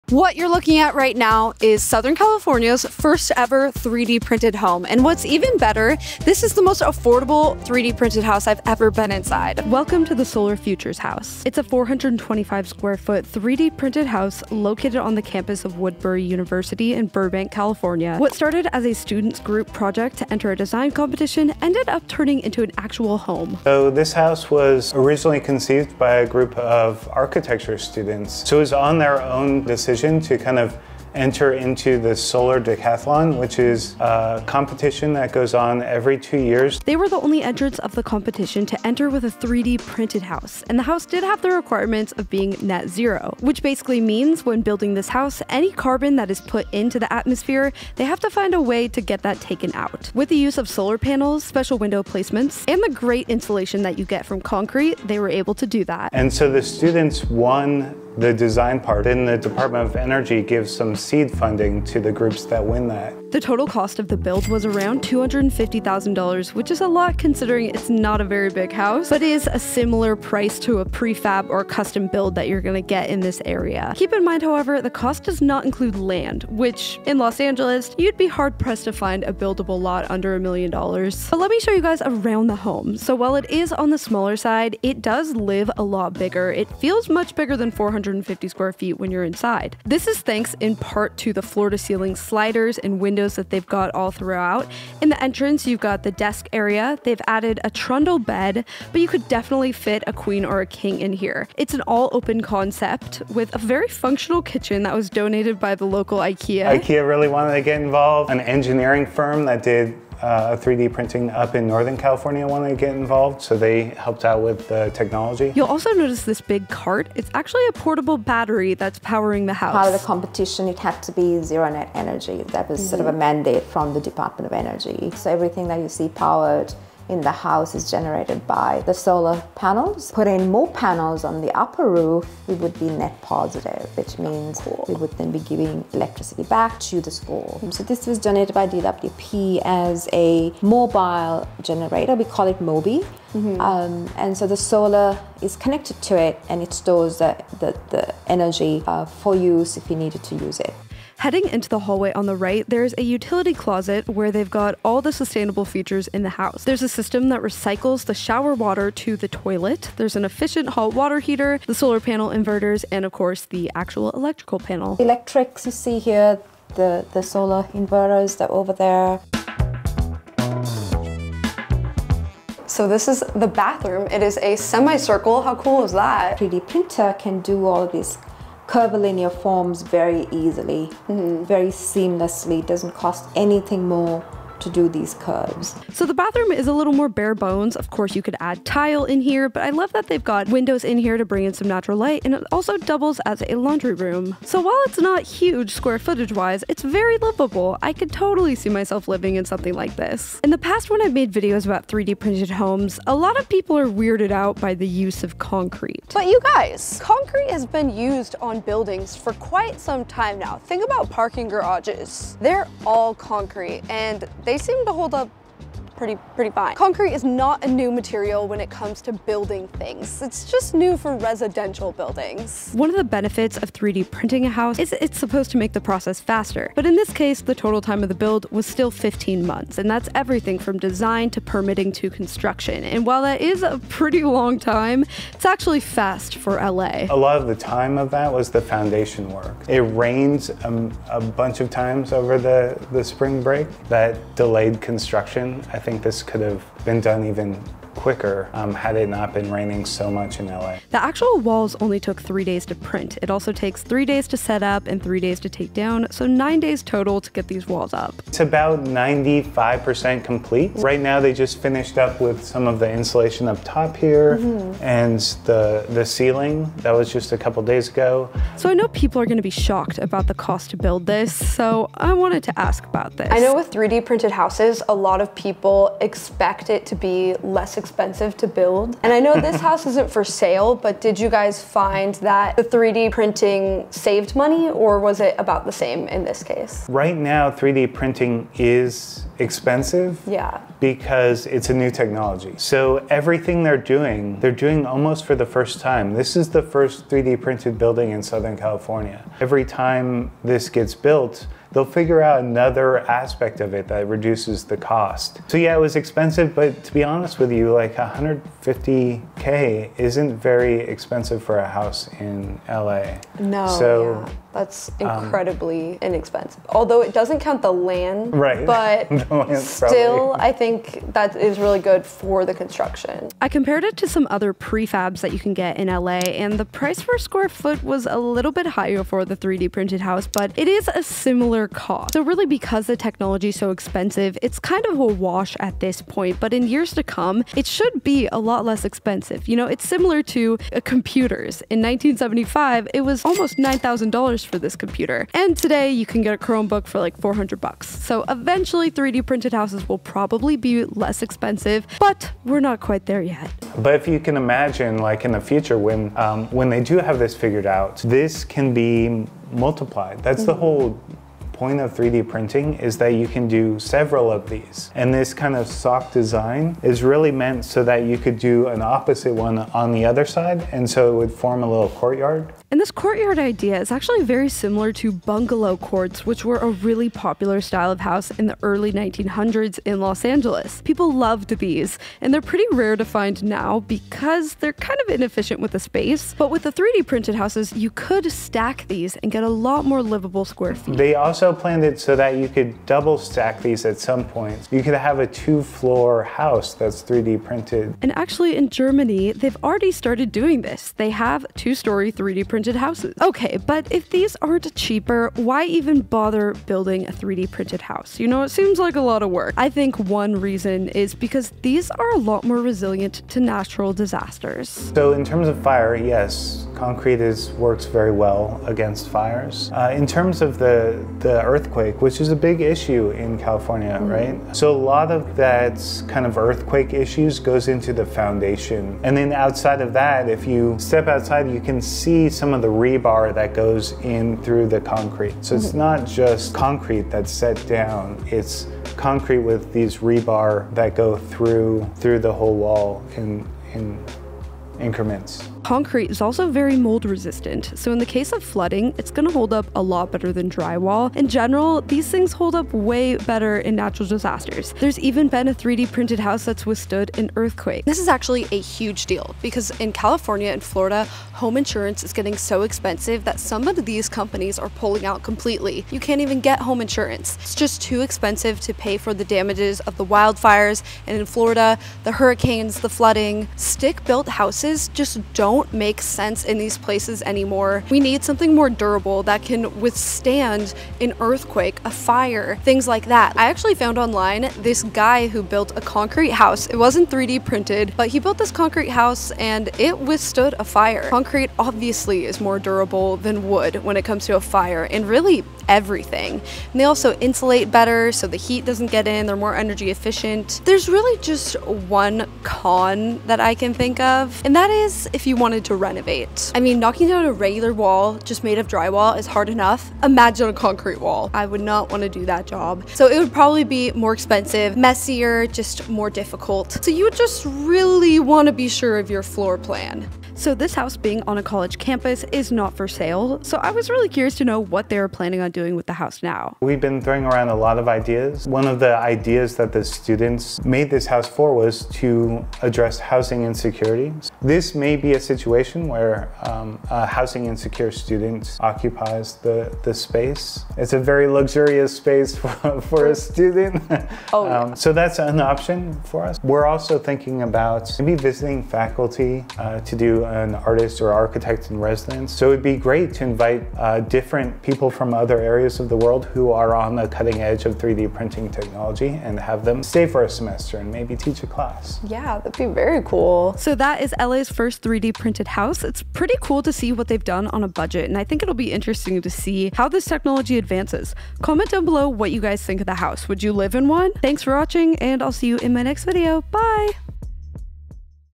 LA’s First $250,000 3D PRINTED House!
Take a look inside Los Angeles' first ever 3D Printed house!

Introduction and Overview of the 3D-Printed House:

 0:01 Origins of The 3D-Printed House Project
1:22 Tour of the 3D Printed House
3:48 Build Time
4:28 Cost vs Traditional Houses
6:50 Benefits of Concrete Houses
8:40 The Plans for This House

This video showcases Southern California's first 3D-printed home, located on the campus of Woodbury University in Burbank. Originally a student group project for a design competition, the house turned into a functional residence. The 425 sq ft house is a Net Zero building, meeting all energy requirements through solar panels and innovative features.

The construction cost was approximately $250,000, excluding land expenses. Despite being a small house, the design and use of 3D printing technology make it feel more spacious. The interior, highlighting features such as a functional kitchen donated by Ikea, a portable battery powering the house, and sustainable elements like water recycling and efficient hot water heating.

While the the current cost of 3D printing technology makes it comparable to traditional construction methods, it notes the potential for future cost reductions as the technology evolves. The house's resilience to natural disasters, energy efficiency, and potential for multi-unit configurations are discussed as advantages. The video concludes by considering potential uses for the house, such as addressing housing insecurities for students or hosting visiting faculty involved in 3D printing technology. Overall, it explores the practicality and benefits of 3D-printed homes in the context of contemporary construction challenges.

What Equipment I Use: